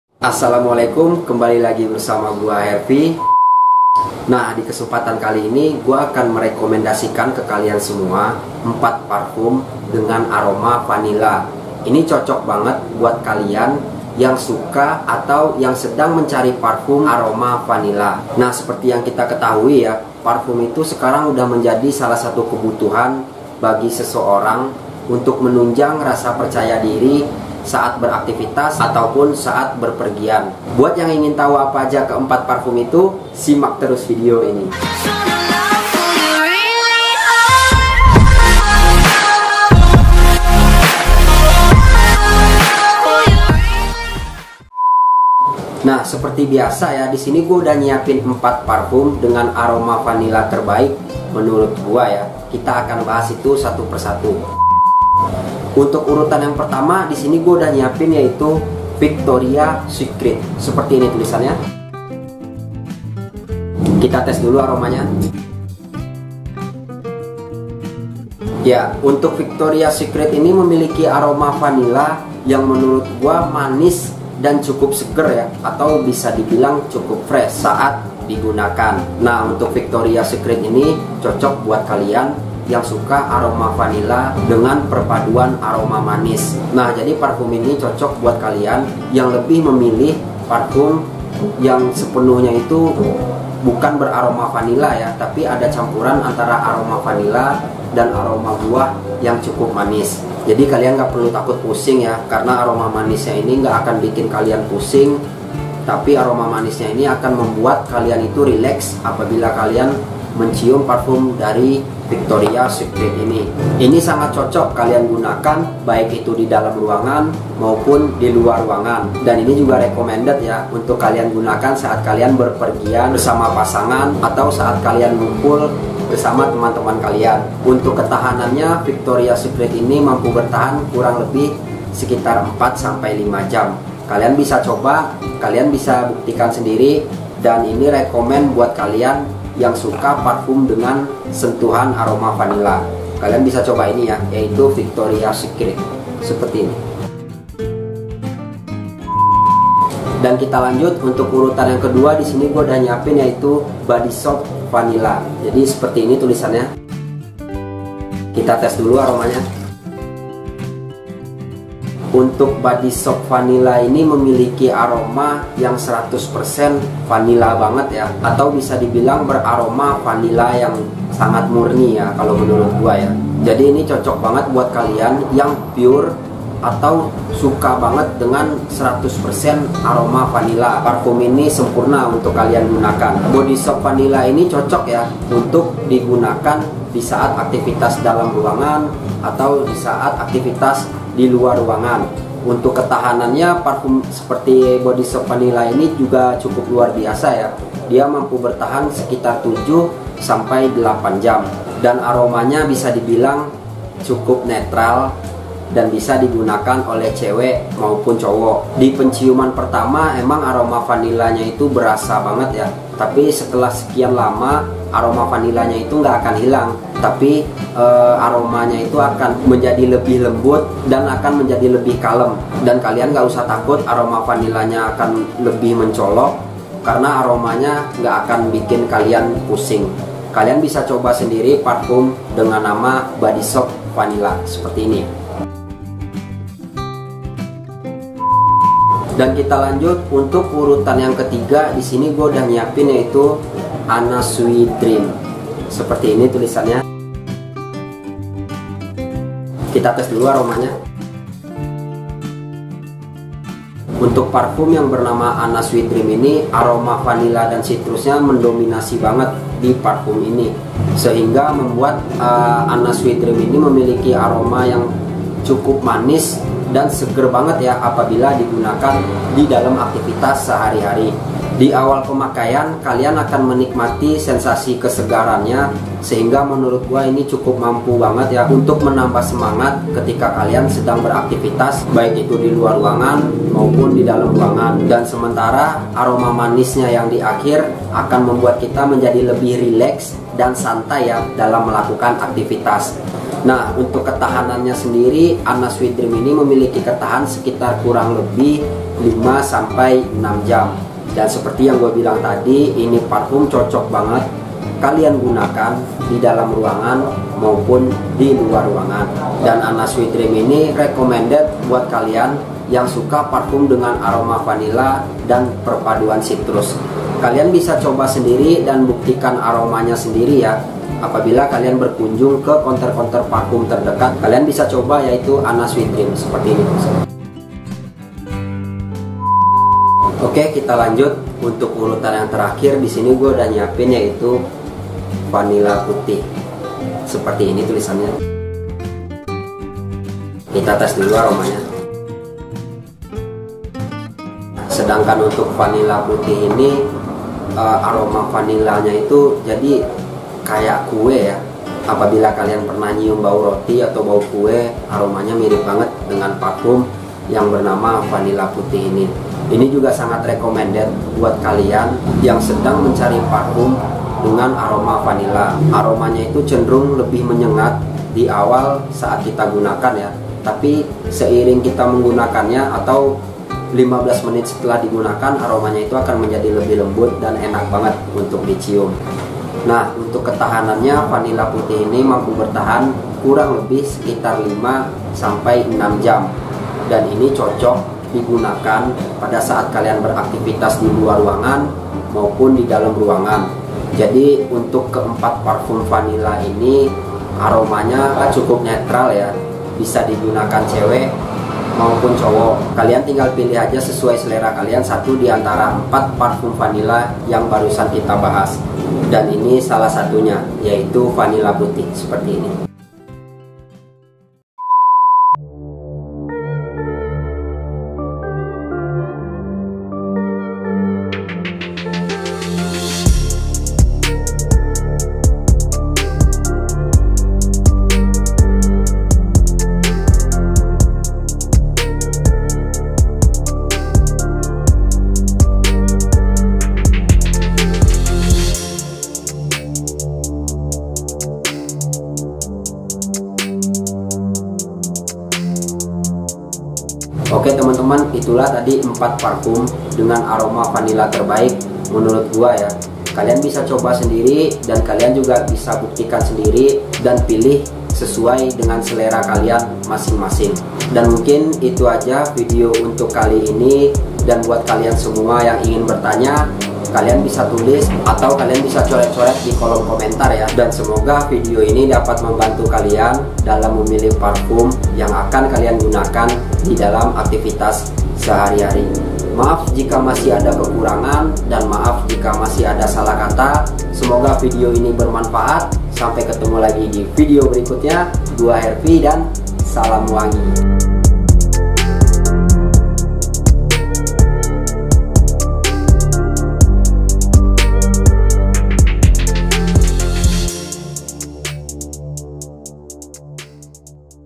MANTAP ❗ PARFUM AROMA VANILLA TERBAIK | parfum ini cocok buat kalian yang suka dengan aroma vanilla
Assalamualaikum bosku semua,kembali lagi di channel bang hervi,pada kesempatan kali ini gua akan rekomendasikan parfum aroma vanila terbaik,ini pas banget buat kalian semua yang suka parfum dengan aroma vanilla,karena parfum dengan aroma vanilla cocok digunakan cewek maupun cowok.
Semoga video ini dapat membantu teman-teman semua dalam memilih dan menentukan parfum yang tepat untuk digunakan dalam aktifitas sehari - hari.
Jika ada yang ingin bertanya silahkan tulis di kolom komentar..

semoga bermanfaat dan selamat menonton..